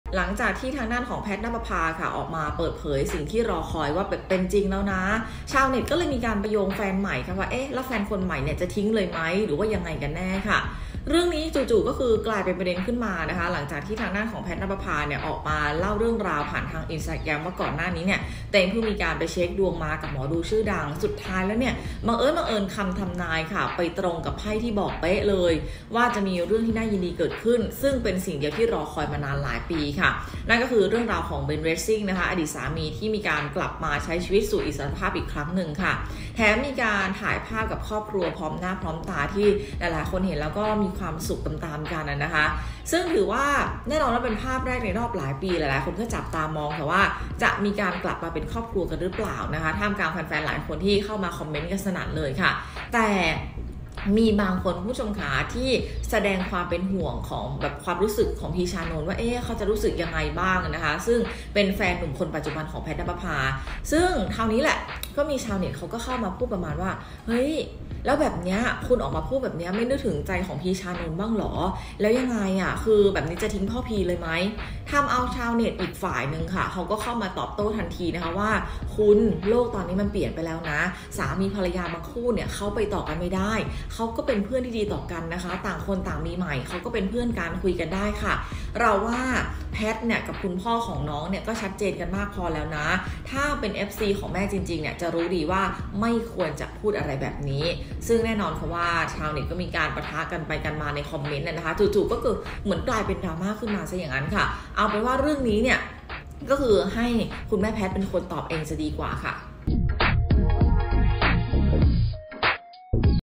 ดราม่าซะงั้น! “แพท” อัดคลิปเผยสิ่งที่รอคอยเป็นจริง จนชาวเน็ตถามแฟนใหม่จะทิ้งเลยไหม? | รู้กันบันเทิง
รู้กันบันเทิง : ดราม่าจนได้! แพท ณปภา เผยสิ่งที่รอคอยเป็นจริง ชาวเน็ตโยงแฟนใหม่ ถามจะทิ้งเลยมั้ย? อีกด้านไม่ทนช่วยโต้กลับ